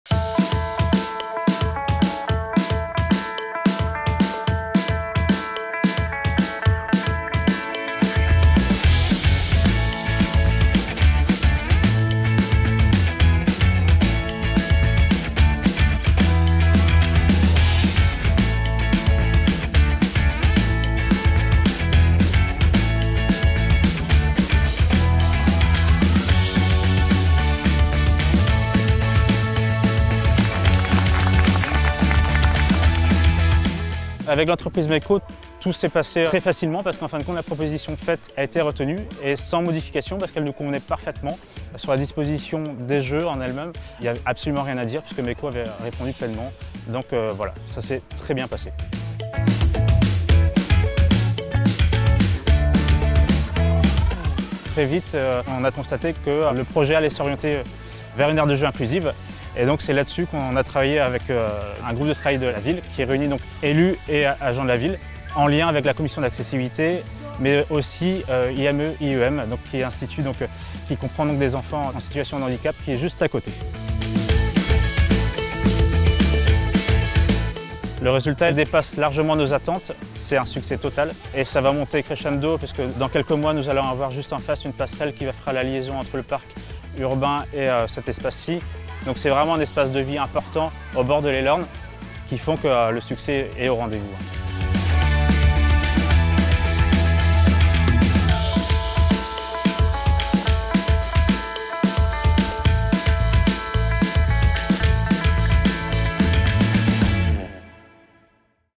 Aire de Jeux inclusive Landerneau, MECO
Nouvelle réalisation Meco, une belle aire de jeux inclusive à Landerneau en bordure de l'Elorn, accessible aux enfants en situation de handicap, le vivre ensemble trouve sa place !